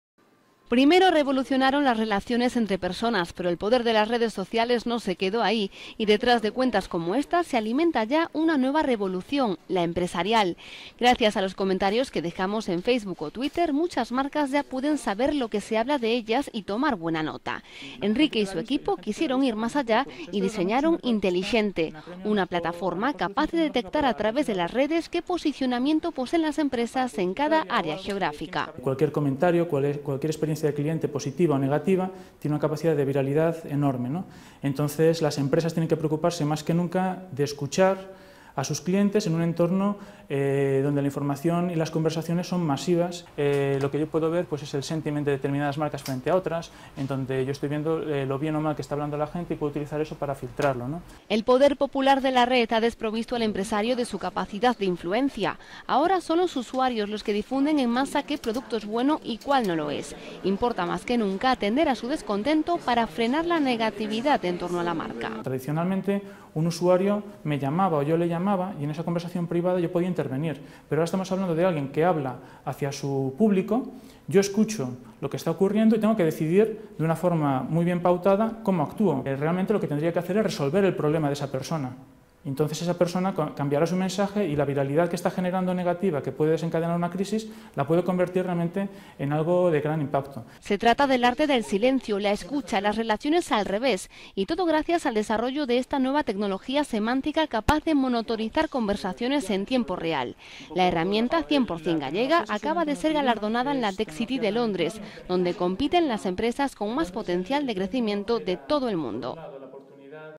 Intellixente: como aprovechar al máximo la inteligencia de las redes sociales
Los comentarios que dejamos cada día en foros, blogs y redes sociales, son los encargados de alimentar a ''Intellixente'', una herramienta informática 100% gallega capaz de detectar en tiempo real el posicionamiento que tiene una empresa, y todo gracias a la información que se desprende de la red. Su creadora la firma 2Mares acaba de ser premiada en la TechCity, una de las citas tecnológicas más importantes del mundo.